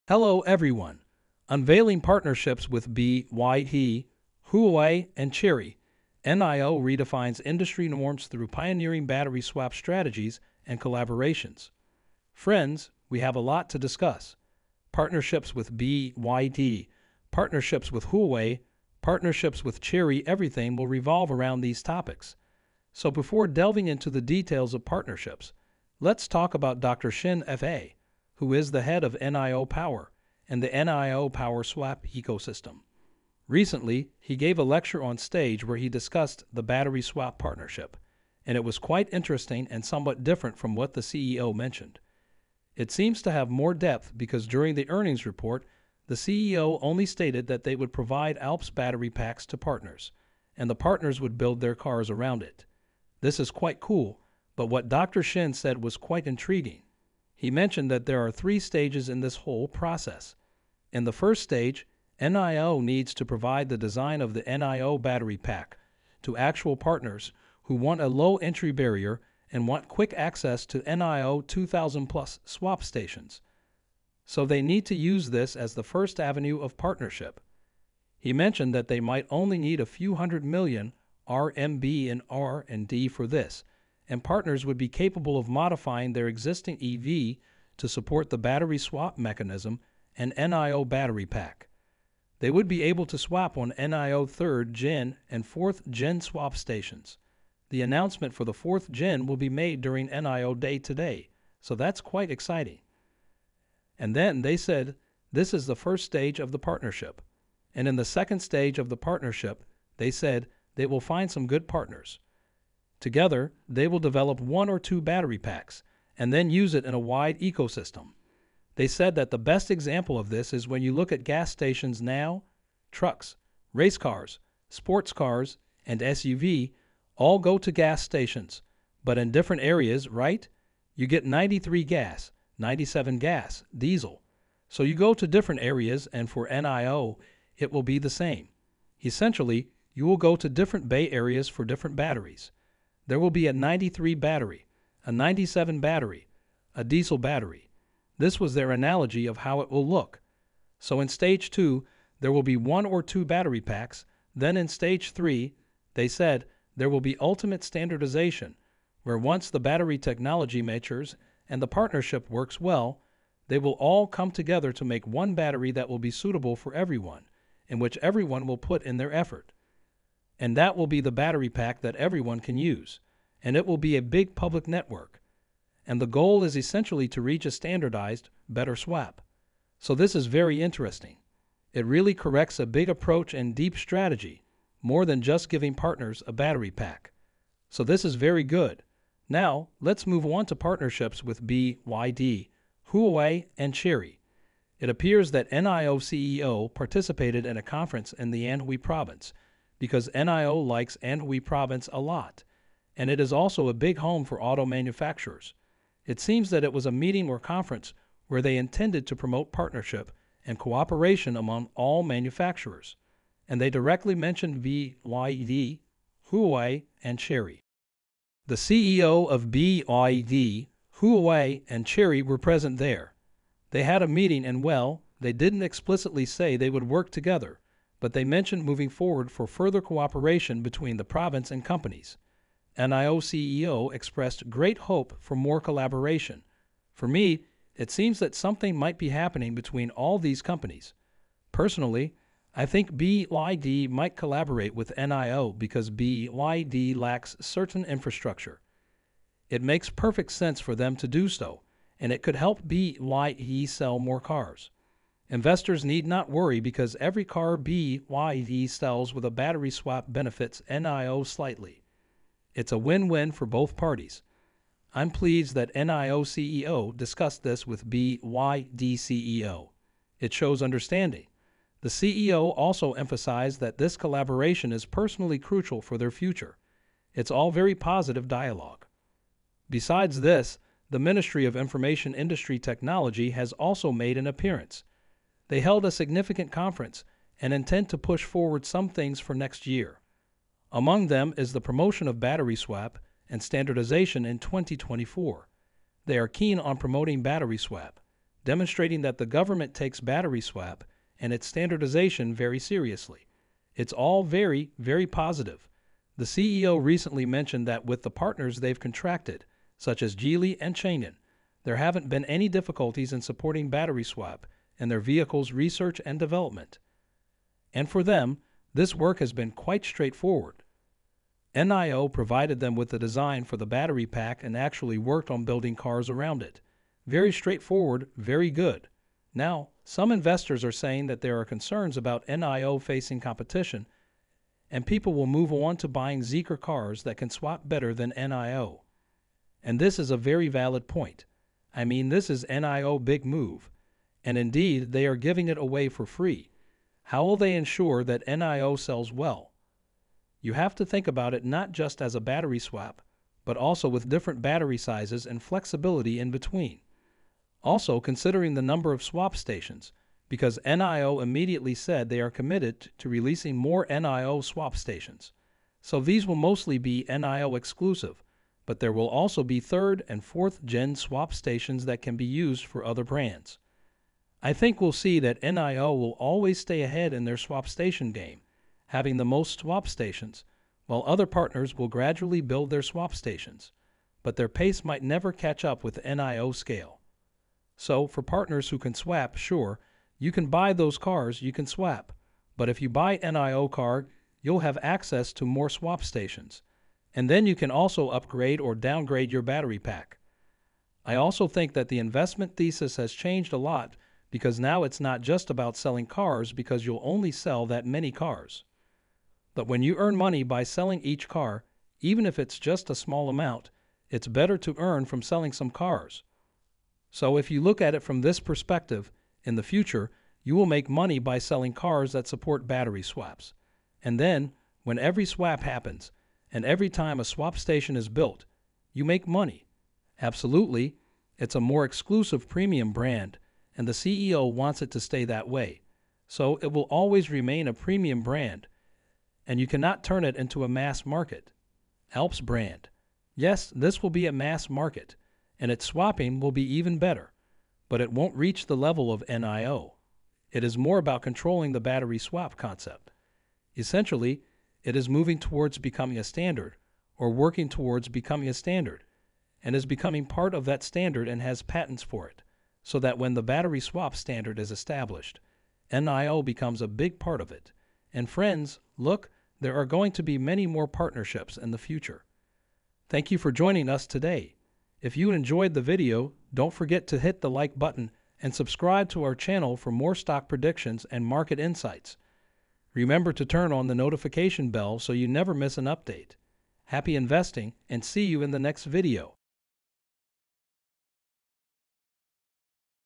NIO Big Step | Exploring Partnerships with BYD, Huawei, and Chery - NIO stock.
NIO redefines industry norms through groundbreaking battery swap strategies and partnerships with BYD, Huawei, and Chery. Dr. Shin FAI outlined a three-stage partnership approach, aiming to share designs, develop specialized battery packs, and establish a universal solution. Discussions at a recent conference hinted at potential collaborations between NIO and key players like BYD, Huawei, and Chery. The government support for battery swap standardization further bolsters this positive trajectory. Despite concerns about competition, NIO vast swap station network and strategic approach position them as a premium brand with a stronghold in the evolving battery swap concept. Their focus extends beyond car sales to a sustainable revenue model tied to each swap, emphasizing their commitment to shaping the future of automotive technology.